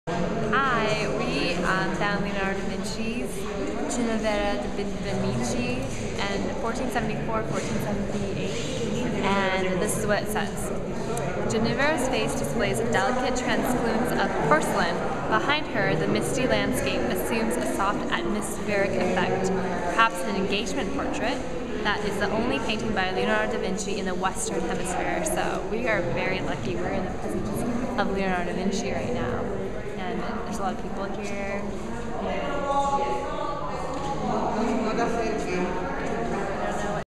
Only Leonardo da Vinci Painting in the Western Hemisphere!
Whoa.

National Gallery of Art in Washington, DC. May 2010.